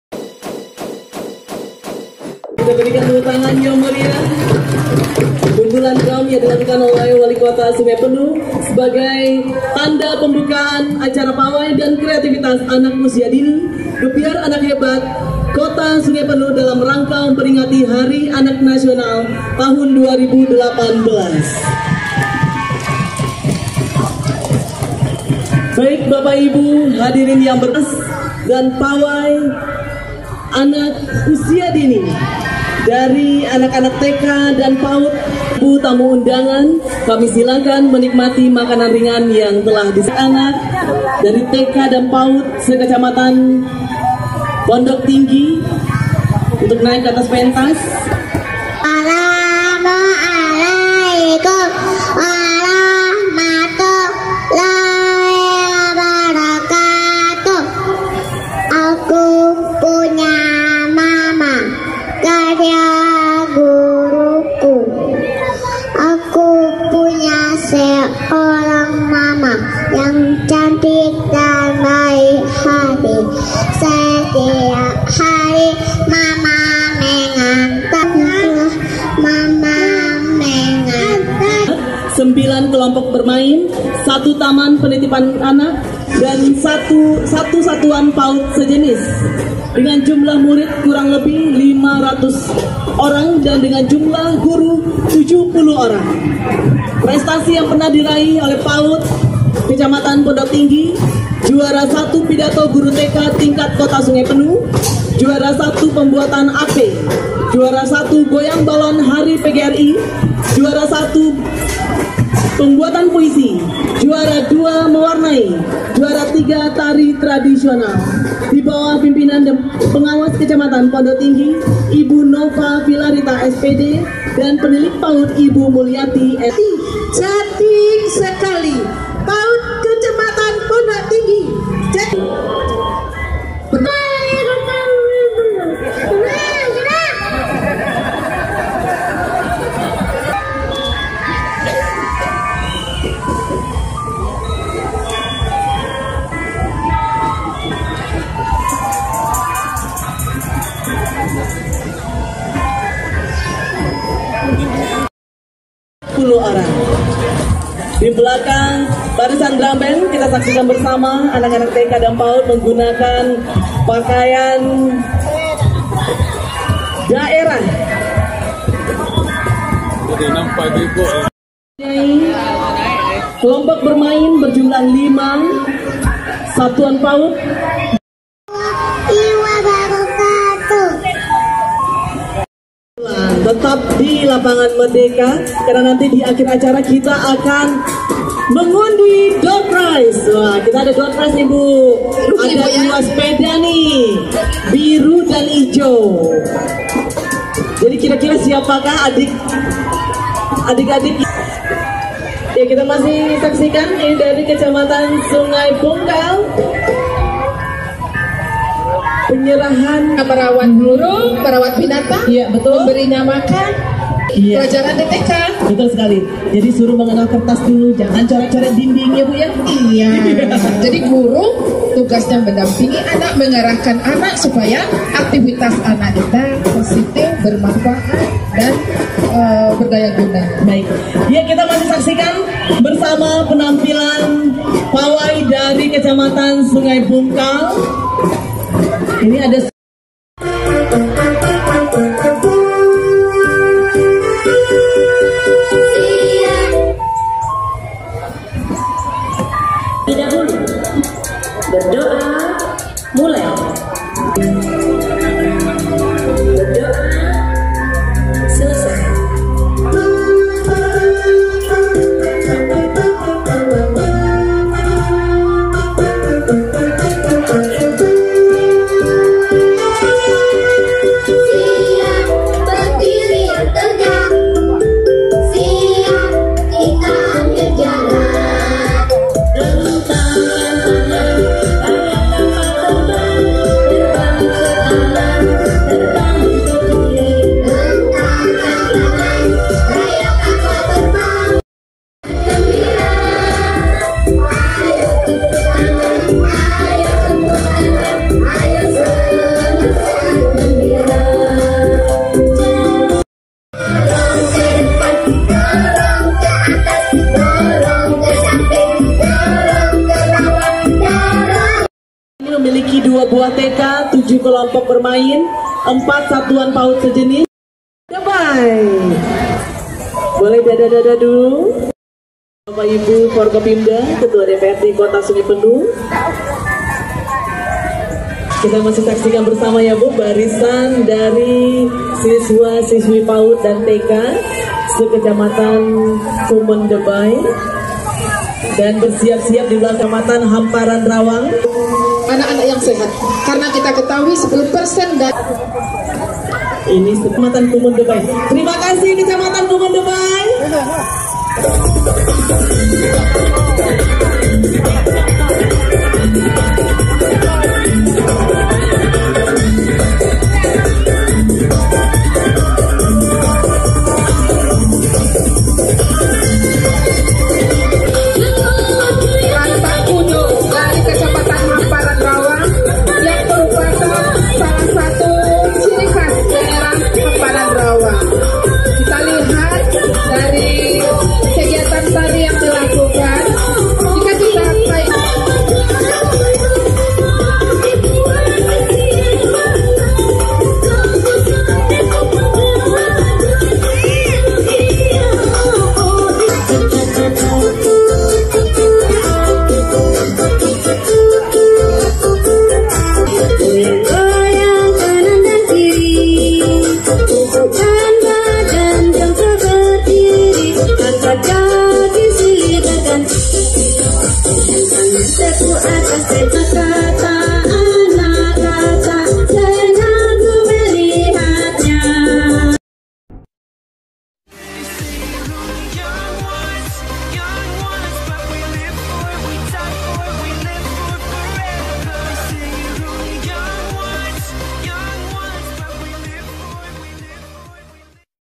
Pawai dan Kreativitas Anak Usia Dini - Gebyar Anak Hebat Kota Sungai Penuh
Acara Pawai dan Kreativitas Anak Usia Dini, Gebyar Anak Hebat Kota Sungai Penuh dalam rangka memperingati Hari Anak Nasional Tanggal 16 September 2018 bertempat di Lapangan Merdeka Kota Sungai Penuh, Jambi.